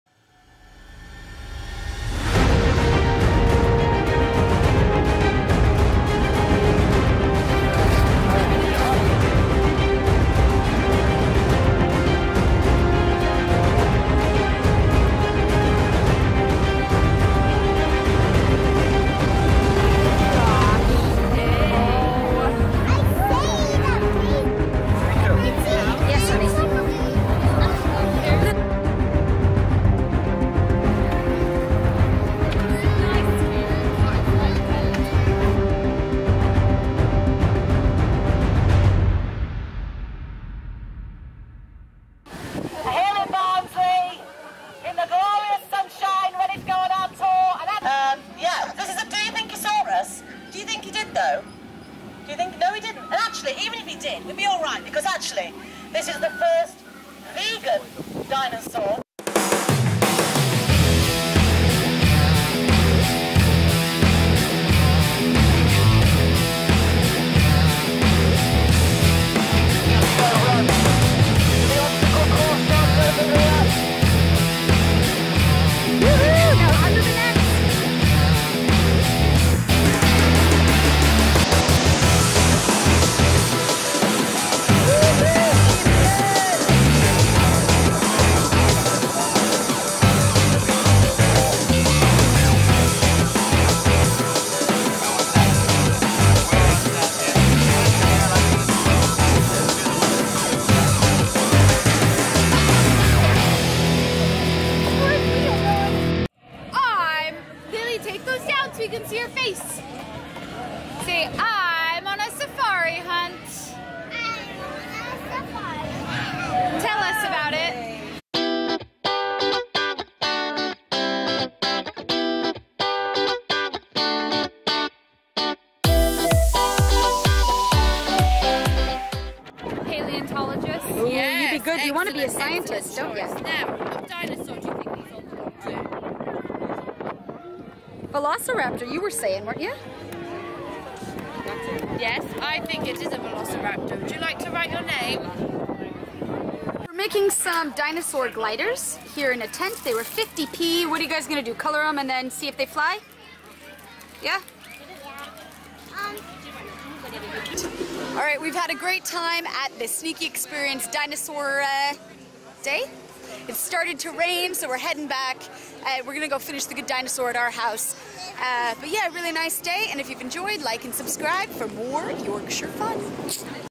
Dino Farm at Cannon Hall Farm
We spent the day with Sneaky Experience at Cannon Hall Farm for a special day of Dino Farm.  We encountered roaming dinosaurs, dug for fossils, rode go-karts, watched outdoor dino cinema and took a dinosaur safari!